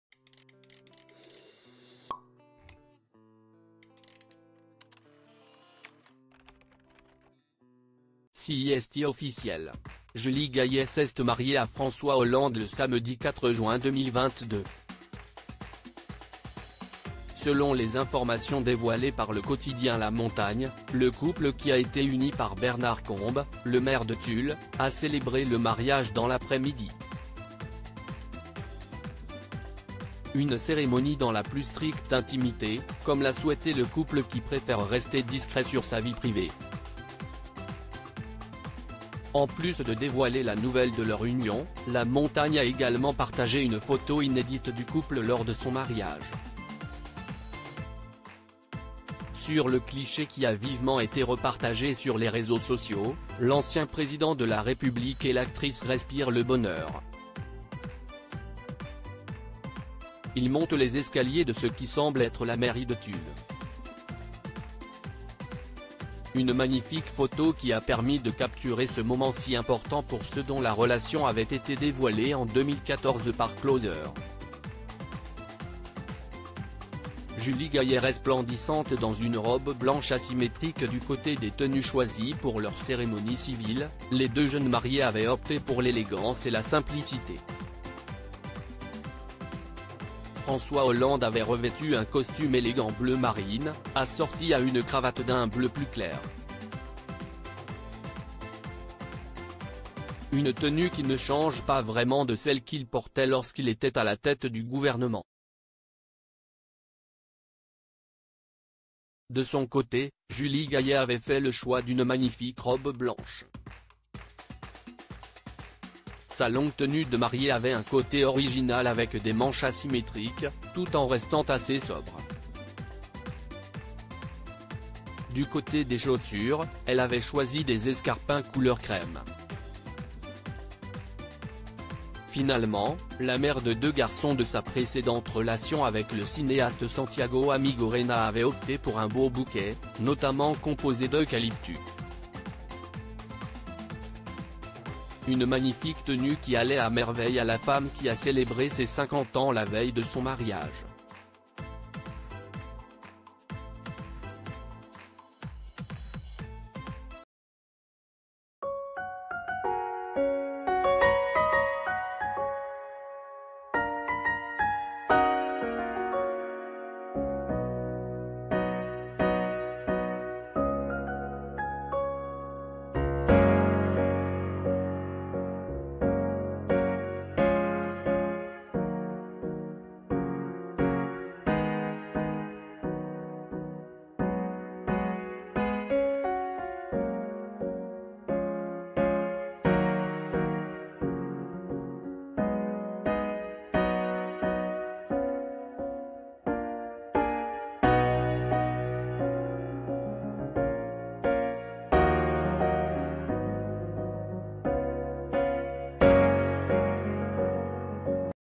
Julie Gayet mariée à François Hollande : robe blanche asymétrique, escarpins pointus et sourire épan
Julie Gayet mariée à François Hollande : robe blanche asymétrique, escarpins pointus et sourire épanoui à la mairie